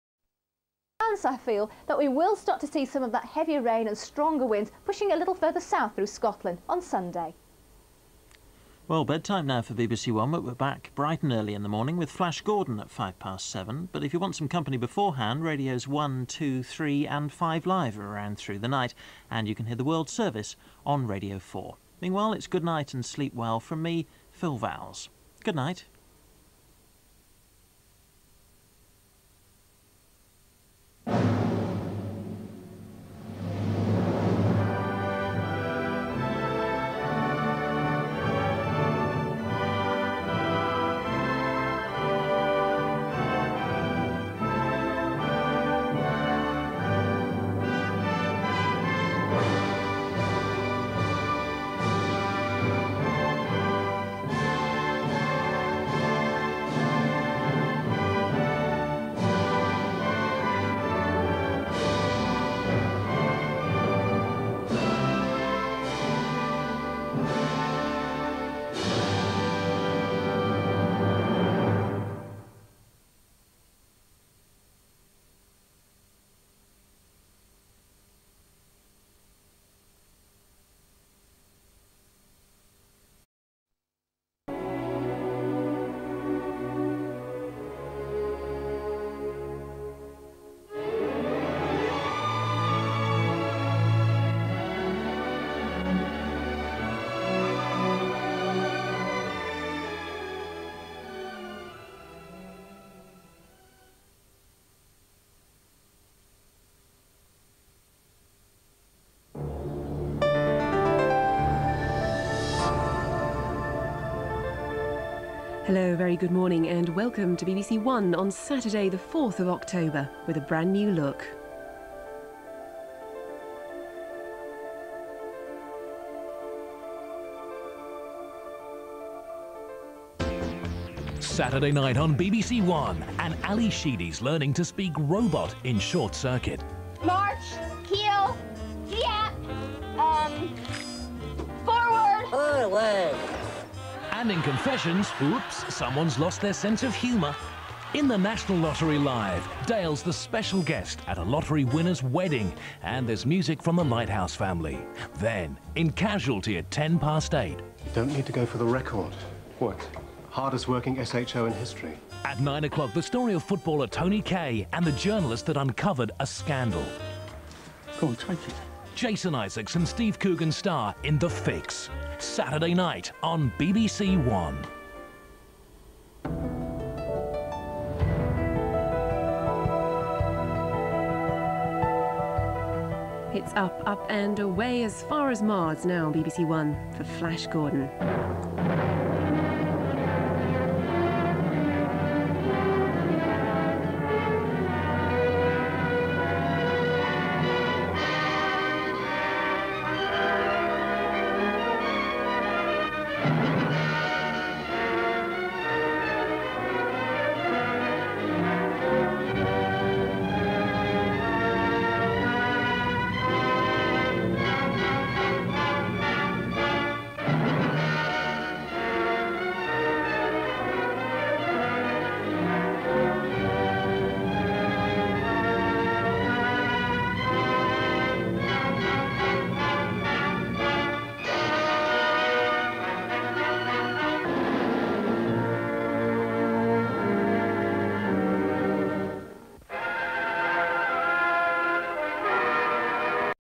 BBC 1 (One) 1997 Rebranding - Final closedown and startup (Balloon idents)
The final BBC1 closedown with the Virtual Globe idents and the first startup with the new BBC One branding, featuring the iconic Balloon Idents designed by Martin Lambie-Narin, who sadly passed away on December 25, 2020.